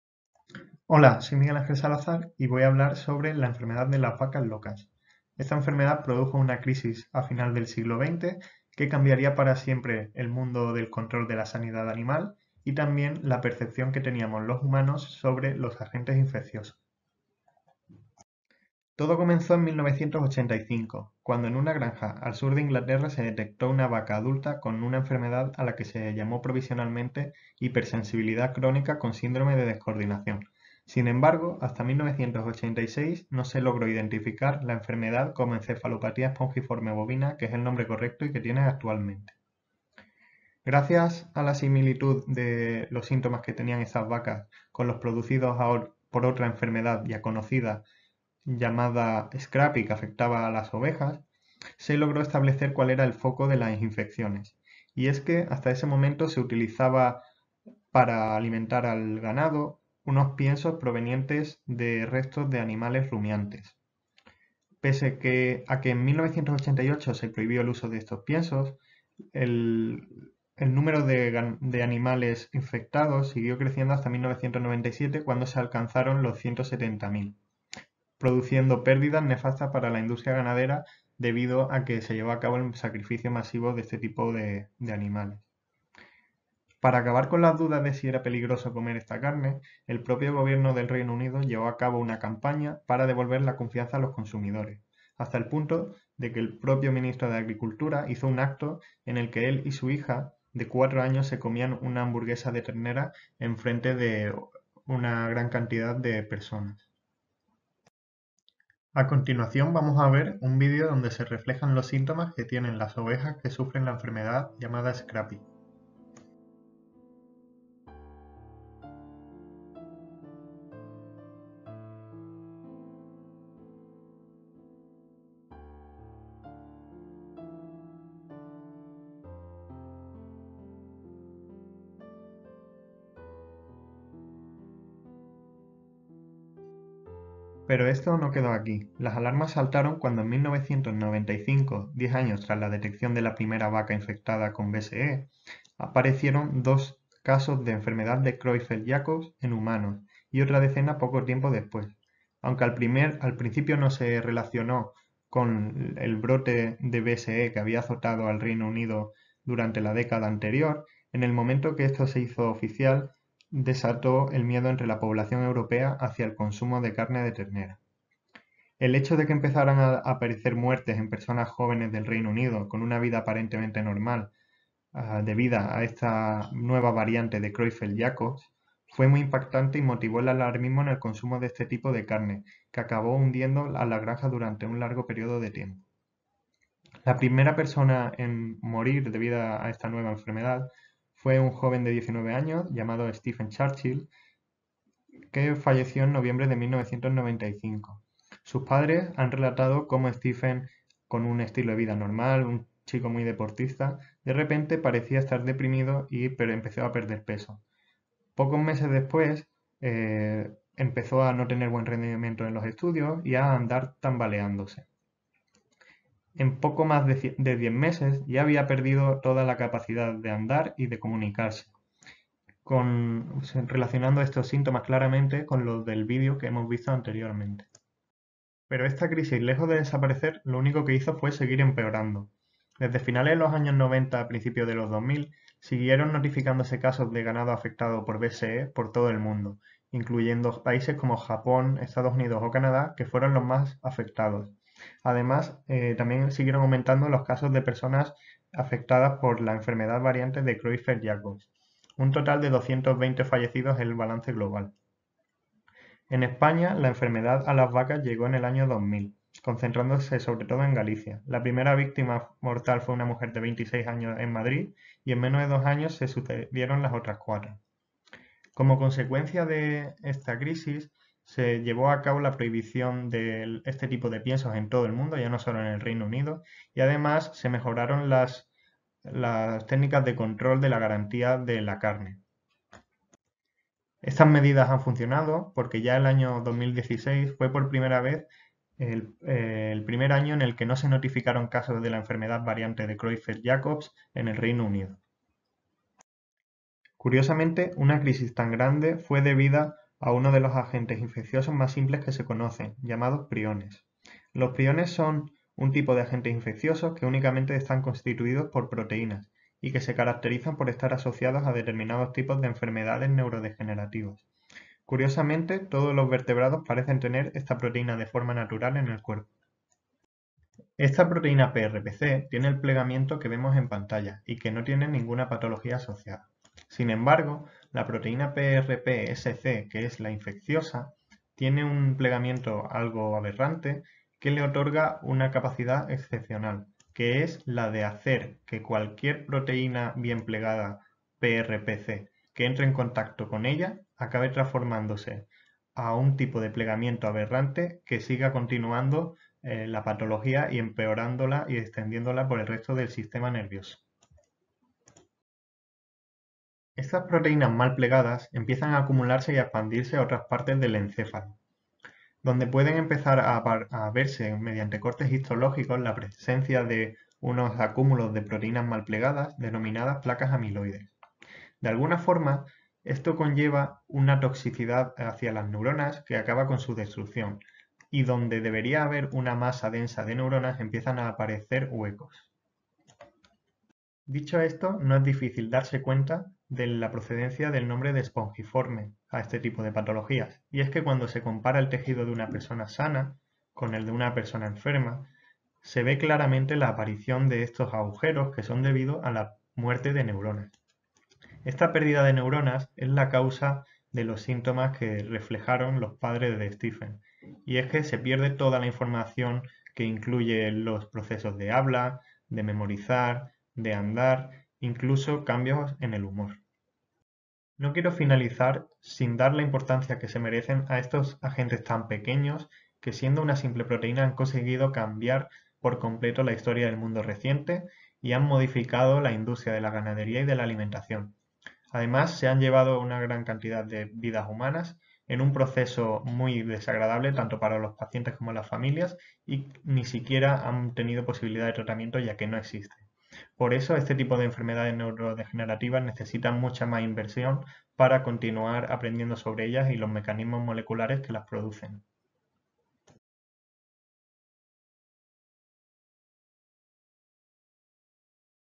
Enfermedad de las 'vacas locas'
Describe cómo una práctica en la industria de preparación de piensos para animales condujo a la expansión de una grave enfermedad que afectó no solo a animales, sino también a personas